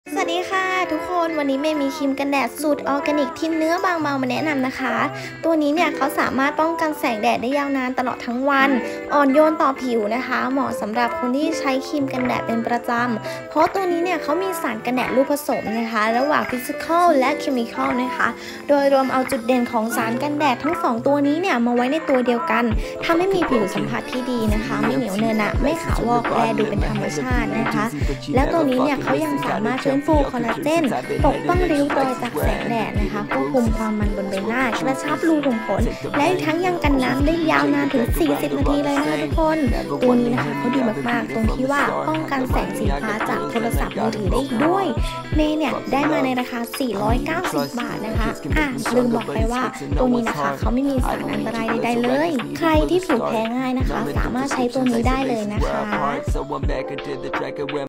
G&T Organic Sunscreen กันแดดออร์แกนิคเป็น Hybrid Sunscreen ที่ผสมระหว่าง Physical และ Chemical
แดดประเทศไทยคือความนัมเบอร์วัน
ออกไปข้างนอกแต่ละทีนึกว่าซ้อมไปนรกค่ะคุณขา
จะ so damn hot อะไรเบอร์นั้น
ได้มีโอกาสลองใช้กันแดดตัวนึงมาค่ะ
นั่นคือ G&T Organic Mild Sunscreen
ซึ่งเป็นกันแดดออร์แกนิคค่ะทุกคน
เป็น Hybrid Sunscreen
ที่ผสมระหว่าง Physical และ Chemical
สัมผัสคือดจีย์มากก ไม่เหนียวเหนอะหนะเลย
ทาแล้วไม่ขาววอก ให้ผิวดูธรรมชาติสุด
แถมยังกันน้ำได้ด้วยนะ
ป้องกันแสง UVA, UVB
และยังป้องกันแสงสีฟ้าจากการเล่น Social Media ต่างๆ
รวมถึงแสงหลอดไฟฟลูออเรสเซนส์
รวมไปถึงให้การบำรุงผิวหน้าไปในตัว
เพราะจะมีส่วนผสมของ Hyaluronic, Vitamin E B5,
Picea Abies Extract, Snow Algae
ช่วยเรื่องการฟื้นฟูคอลลาเจน
ปกป้องริ้วรอยจากแสงแดด ควบคุมความมัน
และทำให้รูขุมขนดูกระชับขึ้นด้วย
แถมยังอ่อนโยนต่อผิวหน้ามากๆ
ใช้กันแดดตัวนี้คือมั่นใจได้เรื่องความปลอดภัยได้เลย
เพราะได้รับการรับรองจาก อย. ว่าเป็นผลิตภัณฑ์ออร์แกนิค
และยังไม่มีสารไม่พึงประสงค์ต่างๆ เช่น Mineral Oil, แอลกอฮอล์, พาราเบน, น้ำหอม, สีสังเคราะห์
เป็นกันแดดที่อ่อนโยนต่อผิว แถมยังให้การบำรุง
ตอนนี้คือใช้ทุกวันเลย เลิฟมากก
ก่อนออกไปไหน อย่าลืมทากันแดดกันนะคะสาวๆ 😉✨

ใครสนใจ ติดต่อไปได้เลยนะคะในเพจหรือซื้อทางShopeeก็ได้ค่ะตามลิงค์นี้เลย